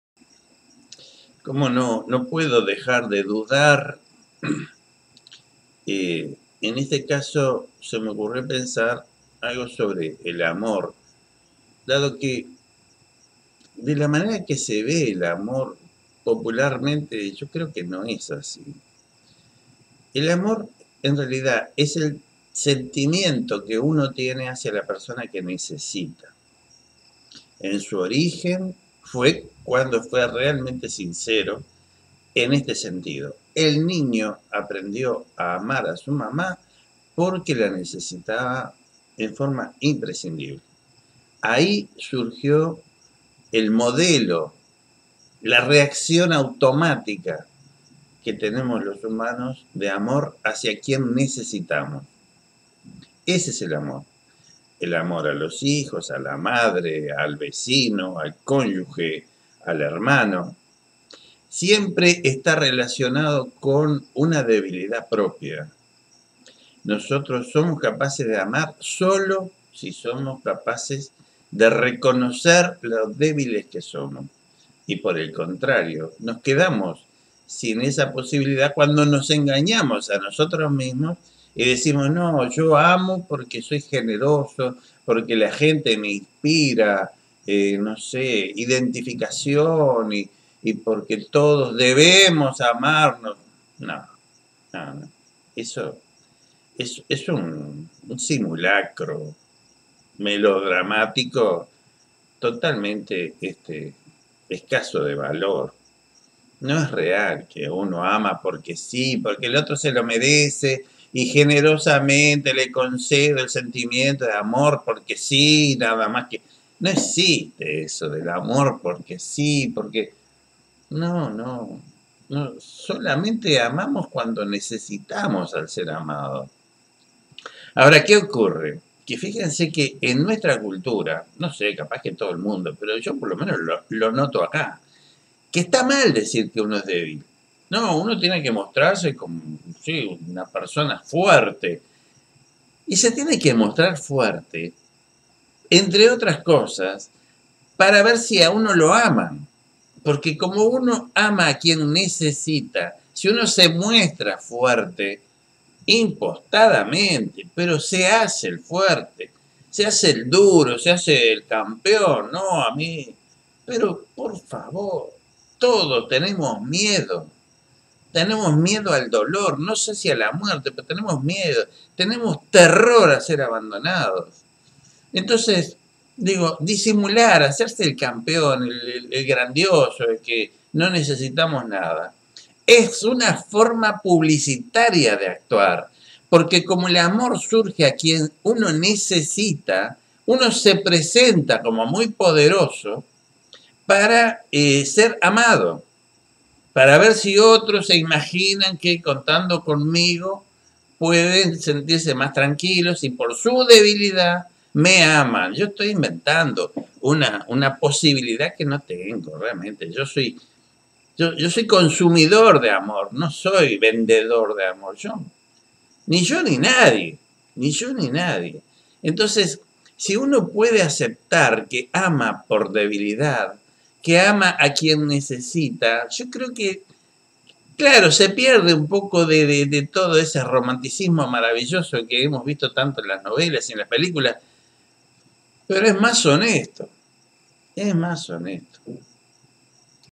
El amor surge de la debilidad
Aparentamos ser fuertes buscando que otros necesiten de nosotros y nos amen. Este video está vinculado a un texto que se publica